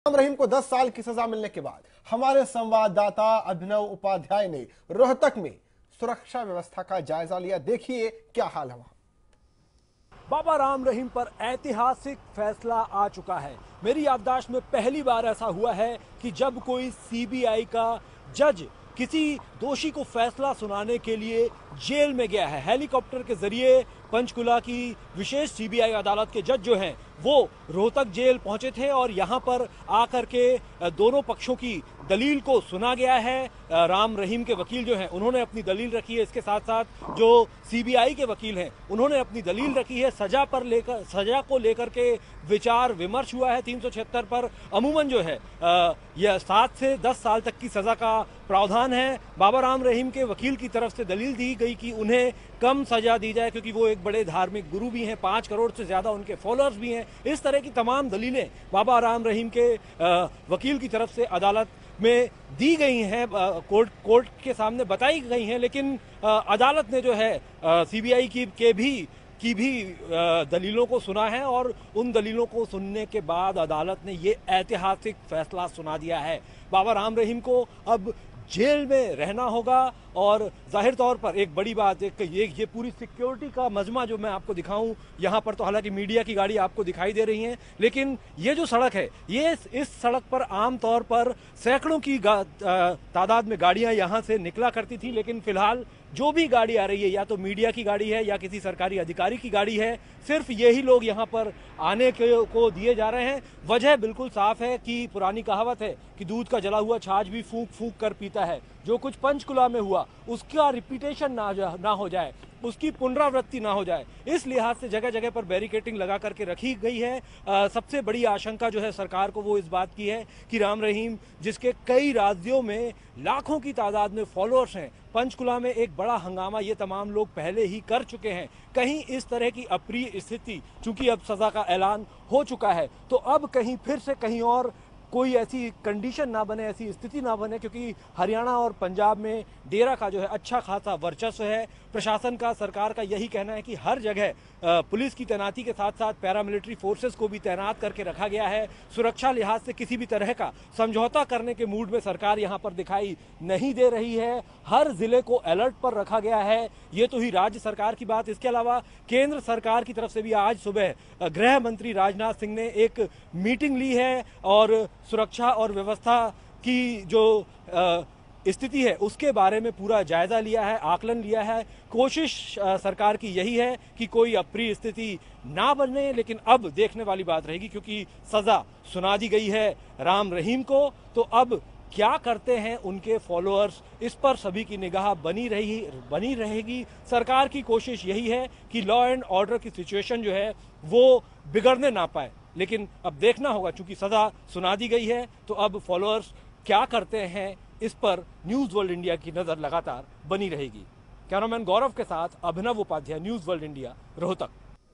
Rohtak On High Alert For Ram Rahim's Quantum Of Punishment
Ram Rahim has been kept in Rohtak jail post his conviction. For his quantum of Punishment, CBI judge fled to Rohtak. Security preparations were made by Rohtak Police to avoid Mayhem.